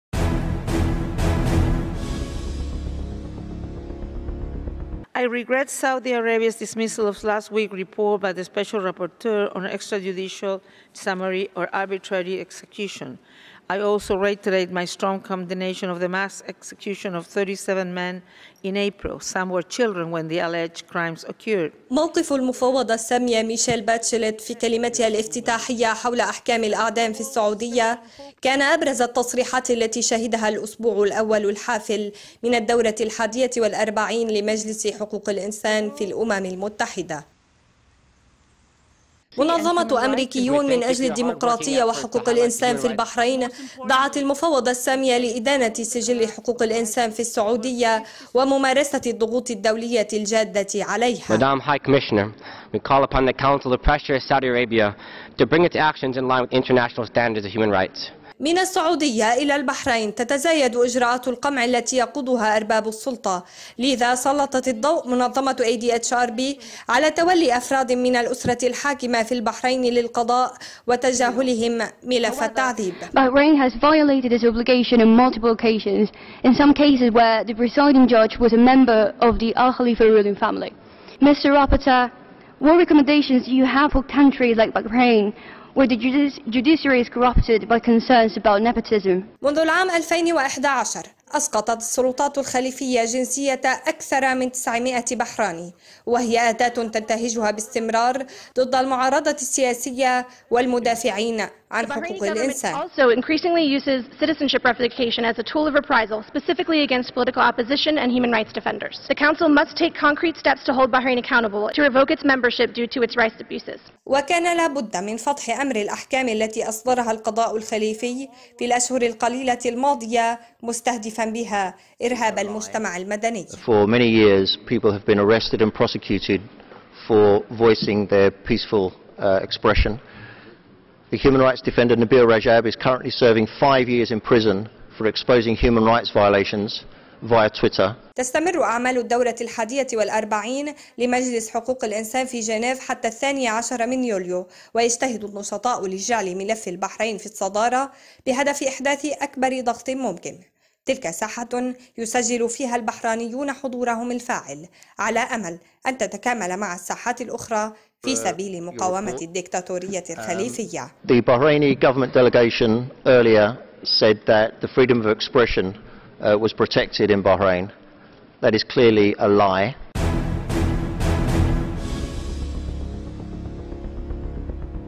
أسبوع حافل بأبرز المواقف المنددة بانتهاكات حقوق الإنسان في دول الخليج البحرين والسعودية والامارات
البحرين اليوم - جنيف ..

موقف المفوضة السامية ميشيل باتشيليت في كلمتها الإفتتاحية حول أحكام الإعدام السعودية كان أبرز التصريحات التي شهدها الأسبوع الأول الحافل من الدورة الحادية والأربعين لحقوق الإنسان في الأمم المتحدة

منظمة أمريكيون من أجل الديمقراطية وحقوق الإنسان في البحرين دعت المفوضة السامية لإدانة سجل حقوق الإنسان في السعودية وممارسة الضغوط الدولية الجادة عليها

من السعودية الى البحرين تتزايد إجراءات القمع التي يقودها أرباب السلطة، لذا سلطت الضوء منظمة ADHRBعلى تولي أفراد من الأسرة الحاكمة في البحرين للقضاء وتجاهلهم ملف التعذيب.

منذ العام 2011 أسقطت السلطات الخليفية جنسية أكثر من 900 بحراني، وهي أداة تنتهجها باستمرار ضد المعارضة السياسية والمدافعين عن حقوق الإنسان.

وكان لا بدّ من فضح أمر الأحكام التي أصدرها القضاء الخليفي في الأشهر القليلة الماضية مستهدفا بها إرهاب المجتمع المدني.

تستمر أعمال الدورة الحادية والأربعين لمجلس حقوق الإنسان في جنيف حتى 12 يوليو، ويجتهد النشطاء لجعل ملف البحرين في الصدارة بهدف إحداث أكبر ضغط ممكن.
تلك ساحة يسجل فيها البحرانيون حضورهم الفاعل على أمل أن تتكامل مع الساحات الأخرى في سبيل مقاومة الديكتاتورية الخليفية